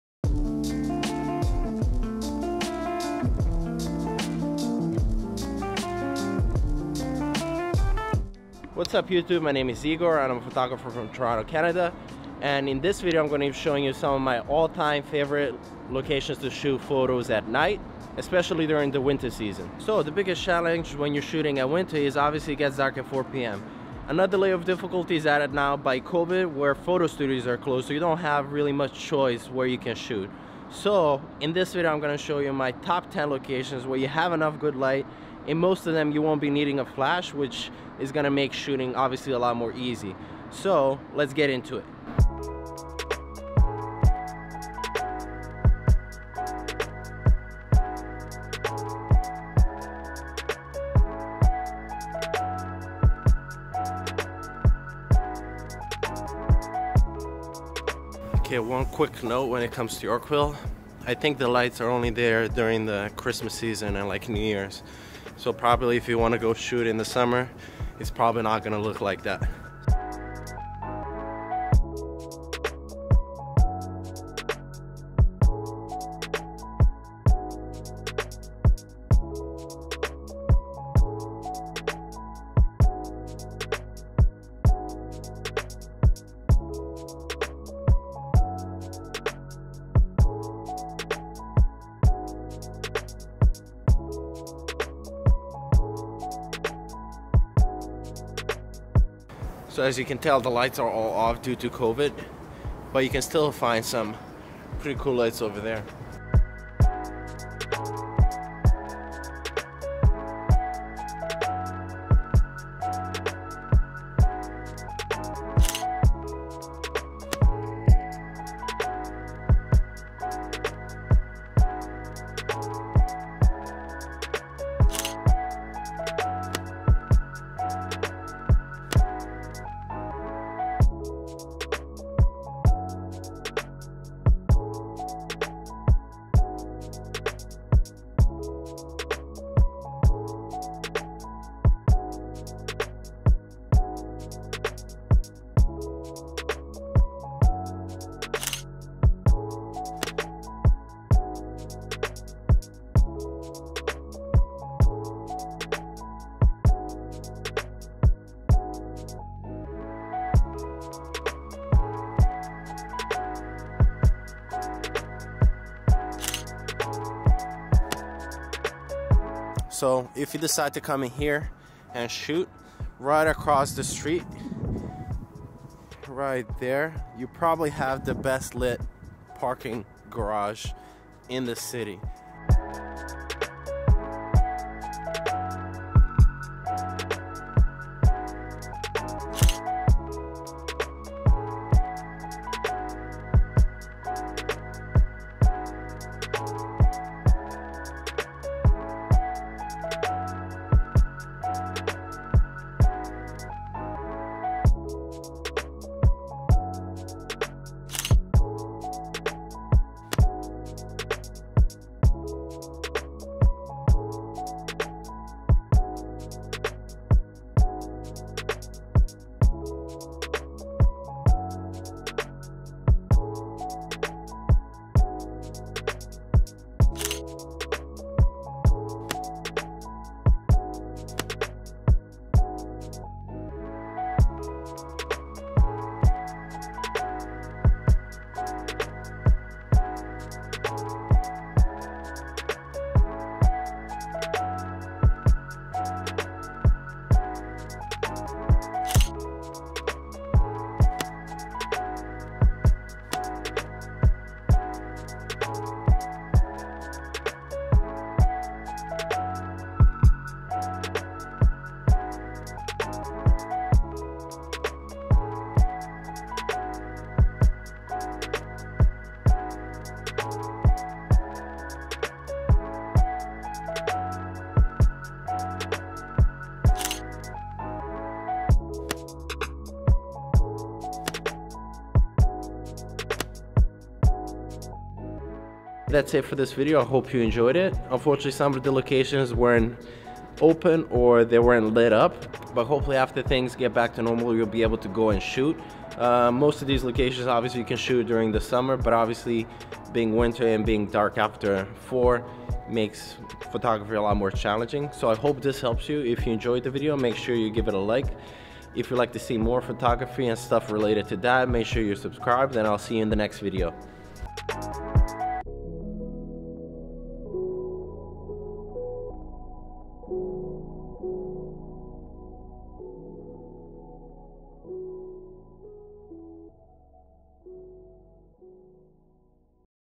Best Night Photography Locations In Toronto
In this video, I will show you the best locations in Toronto, Canada for night time photography.
These are my personal favorite locations in no particular order.

Best Night Photography Locations In Toronto Camera

Lens 1

Lens 2

Lens 3

Lens 4